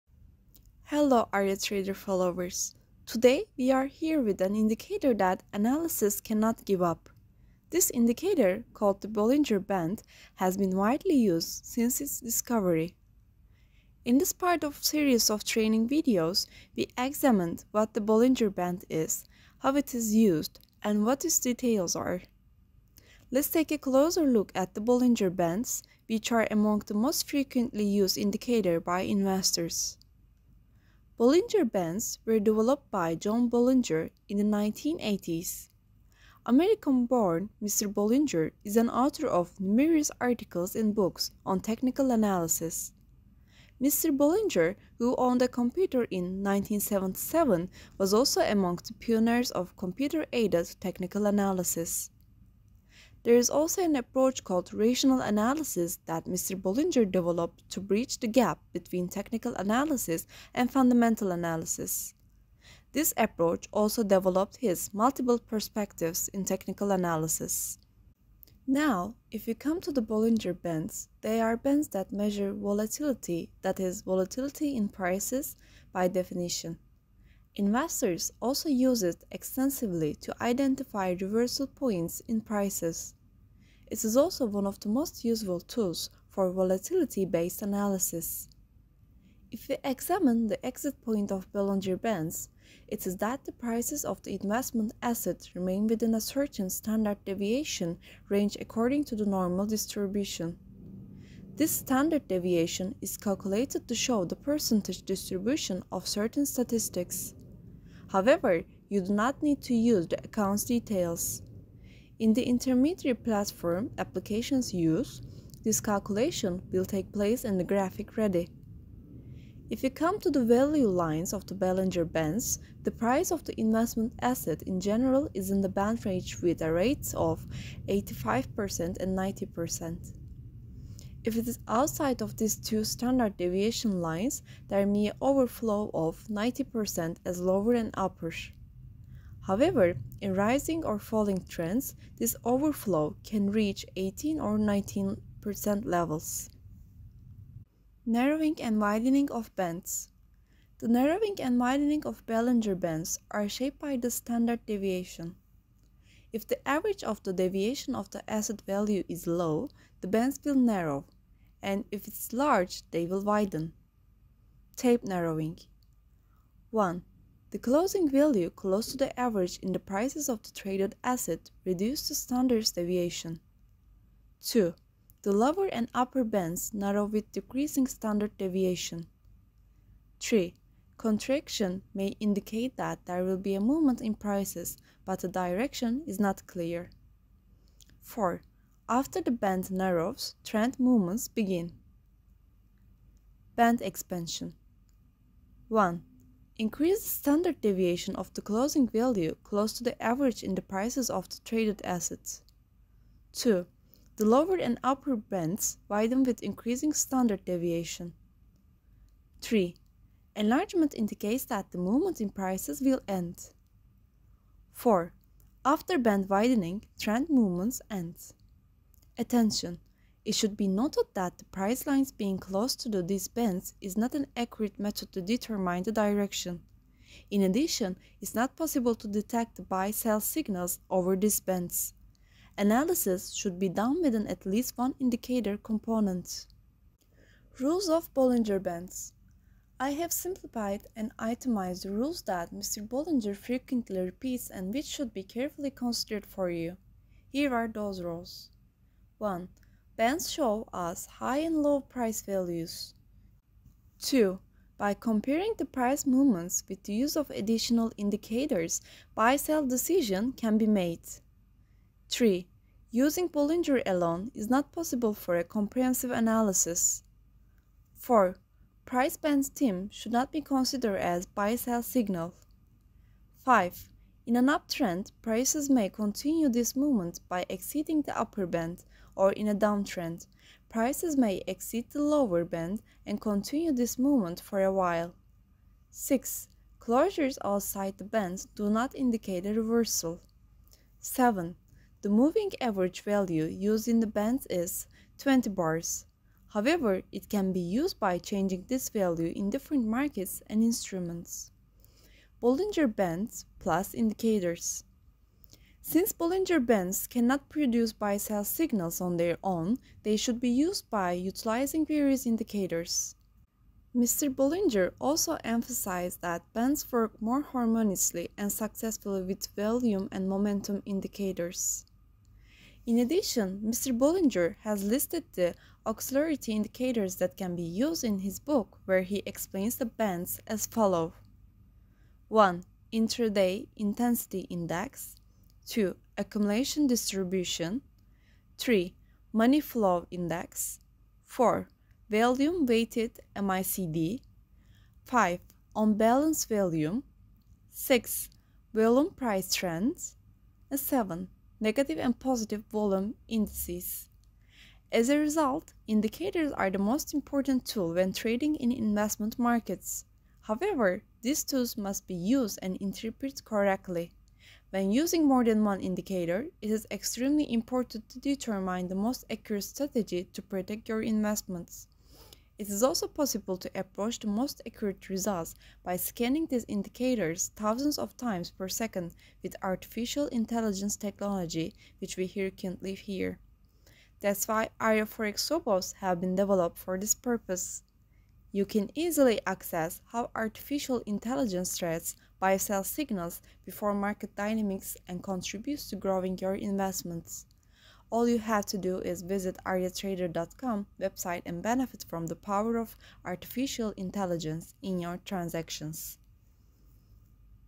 What is the Bolinger Bands? And details of Rational Analysis
You can learn about the trading power in the markets in our video on Bollinger Bands. It is also possible to determine the turning points in prices and to secure your investments with the Bollinger Band strategy. In addition, other questions you will find answers to in this video are as follows;

How to use Bollinger Bands?
How does the Bollinger band work?
What does Bollinger band indicate?
Is Bollinger Bands a good indicator?
When should you use Bollinger Bands?